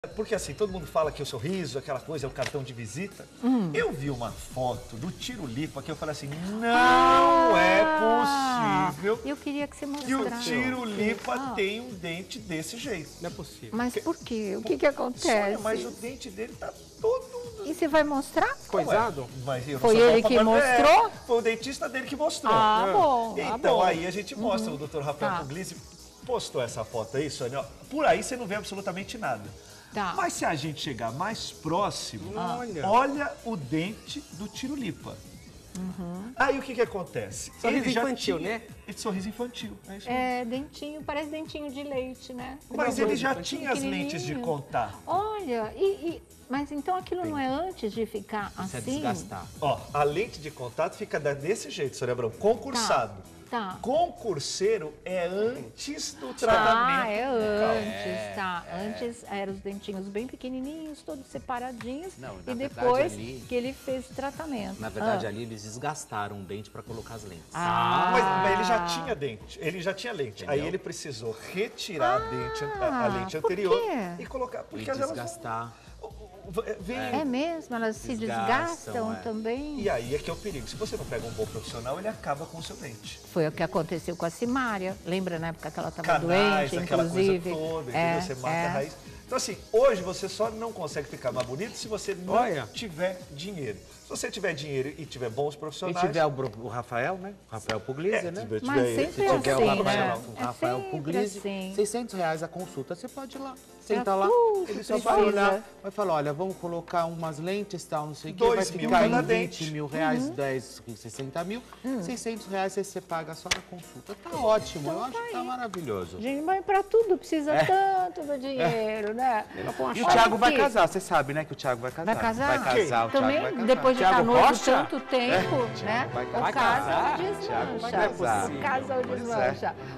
Tirulipa surpreende com antes e depois de "dentes repaginados”
O humorista Tirulipa surpreendeu ao mostrar seu "antes e depois" de seus "dentes repaginados" após tratamento com dentista. Na foto, ele brinca trocando os termos por "concurseiro e concursado”.